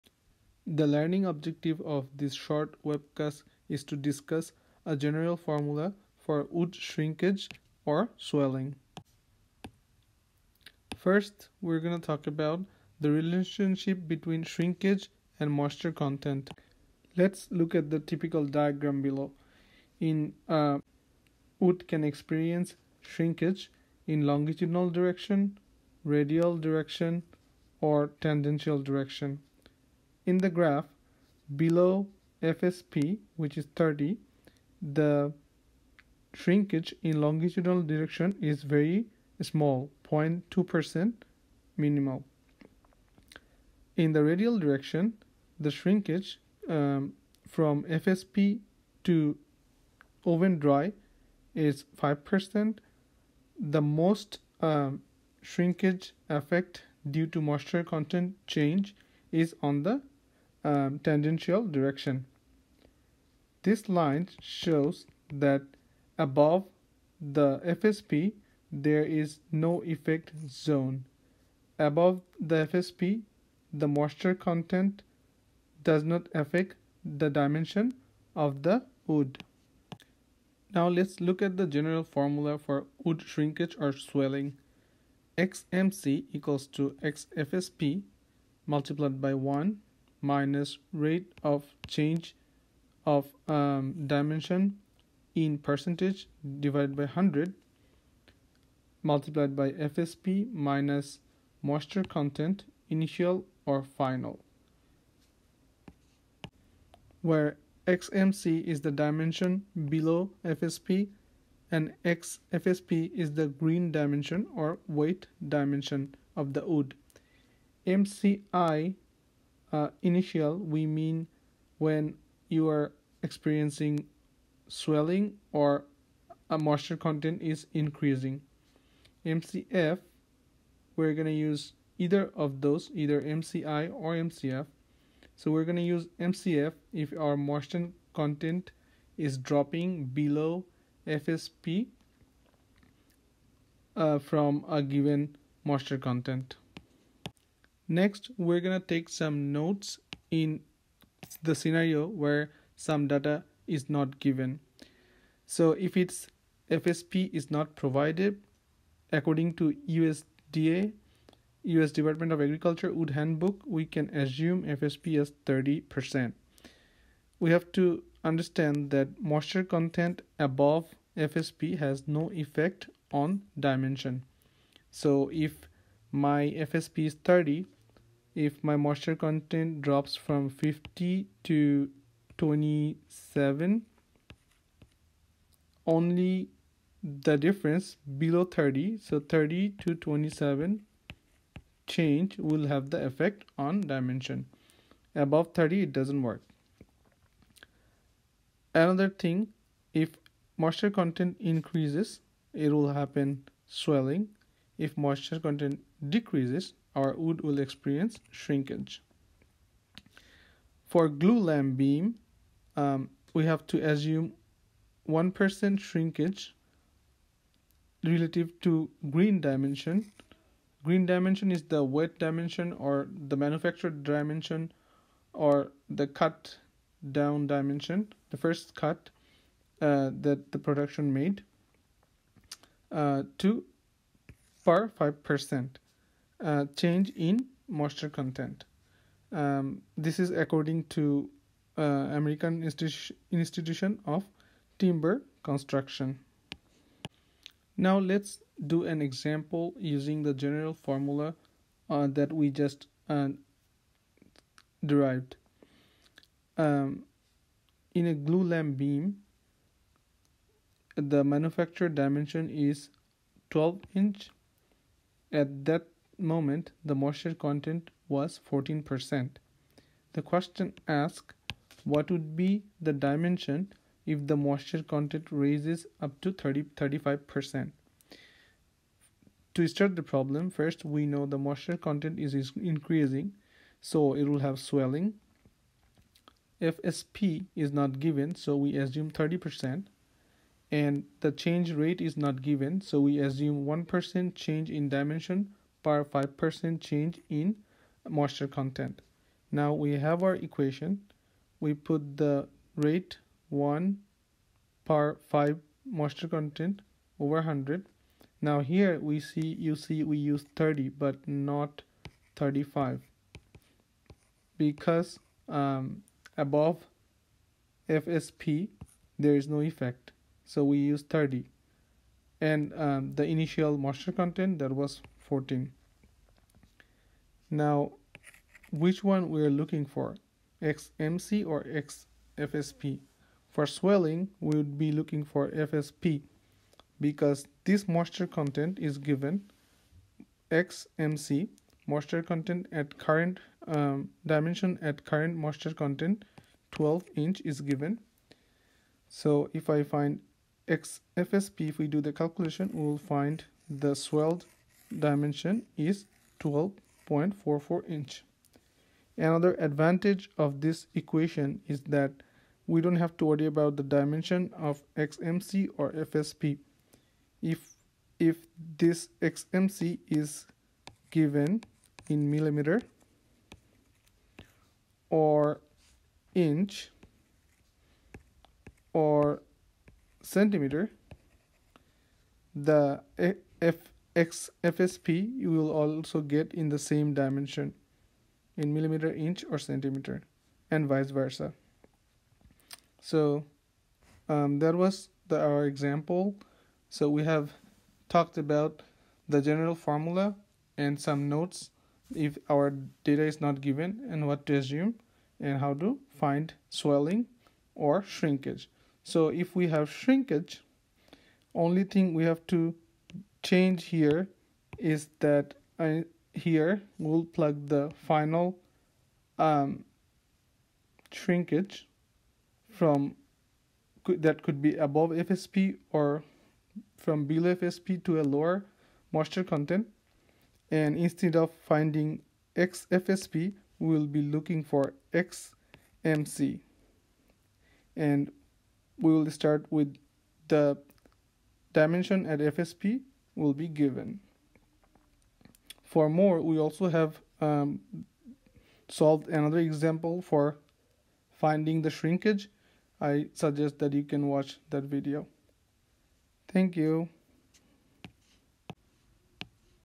A General Formulae to Calculate Wood Shrinkage or Swelling due to Change in Moisture Content
In this short webcast, we discuss a general formula to calculate wood shrinkage or Swelling due to Change in Moisture Content. The notes for assumptions are provided. And problem 10.10 of the textbook by Mamlouk.